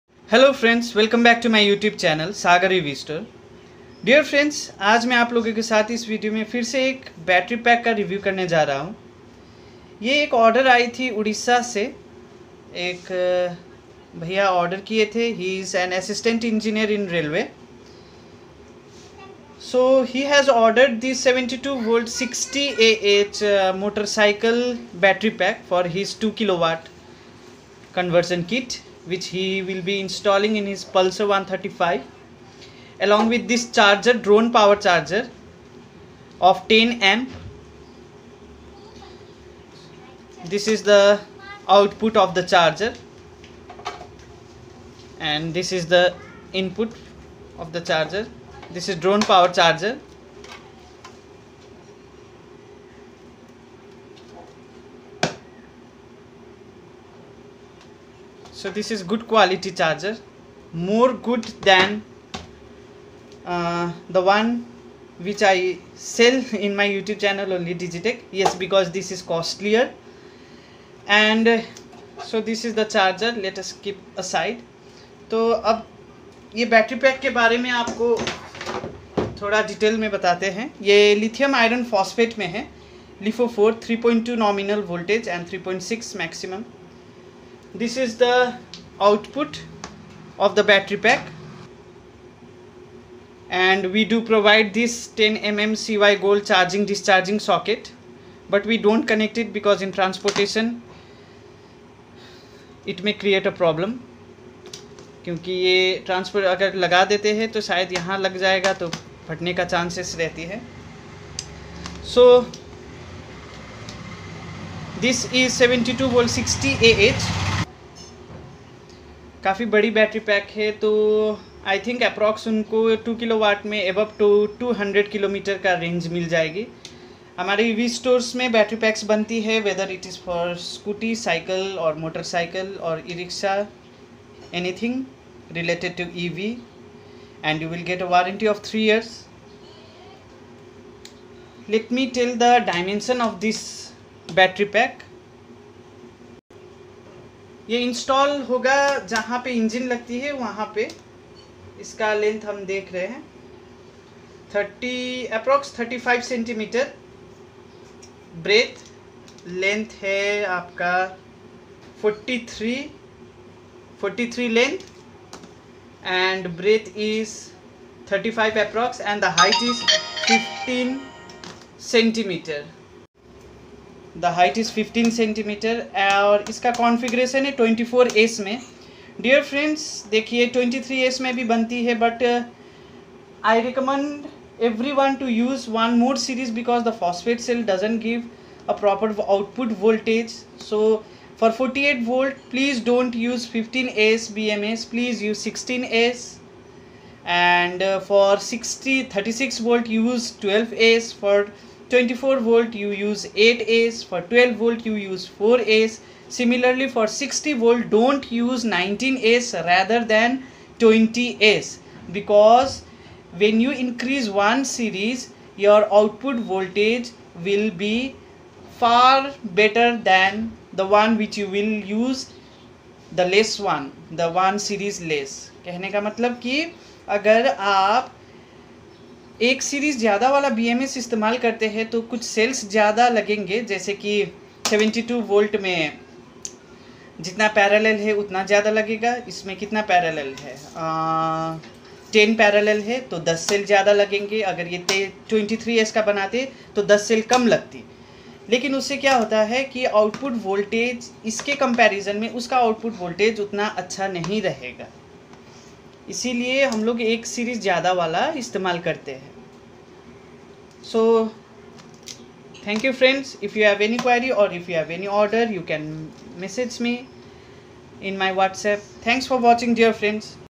Ebike battery for 2kw motorcycle kit in lifepo 72v 60ah.
FOR ANY REQUIREMENT PLEASE WHAT'S APP ME THROUGH THIS LINK

process involved in making battery packs in sagar ev store includes the followings.

1. fresh stock 3c ev grade cells
2. Ir testing
3. spot welding
4. balancing by smart and active balancer
5.Daly bms used
5. use of heat resistance tape all over h type strip
6.caption tape
6. 1mm epoxy sheet use
7.Heat sleeve
8. load testing